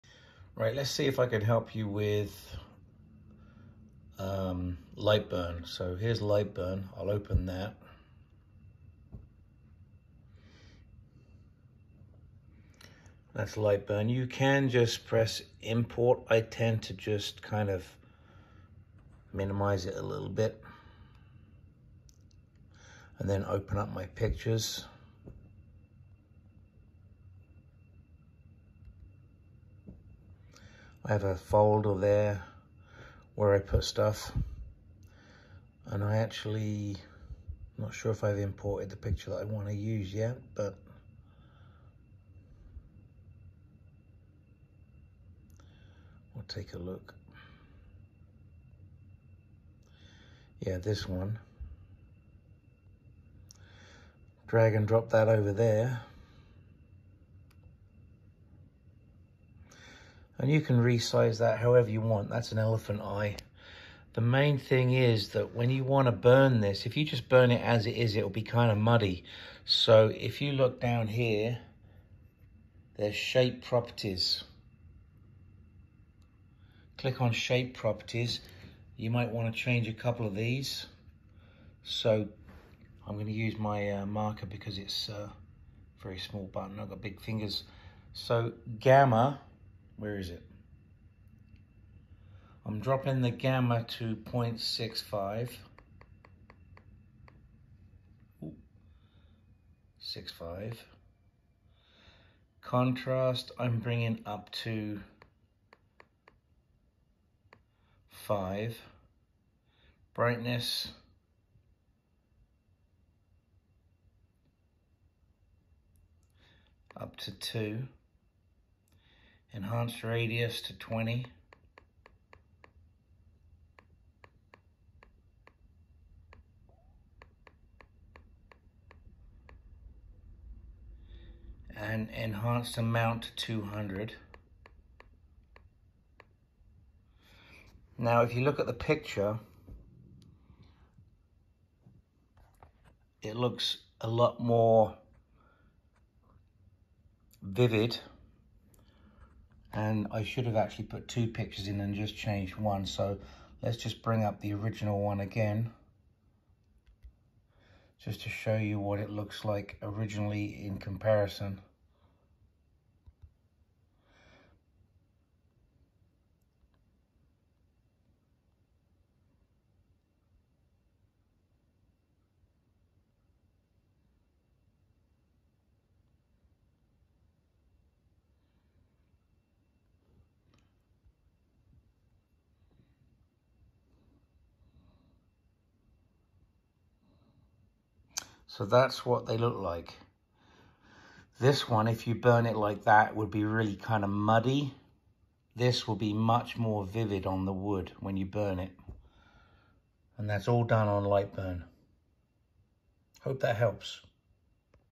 Lightburn settings
Tips on lightburn software
Make your images more sharp and clear when you burn them onto wood by changing a couple of shape properties settings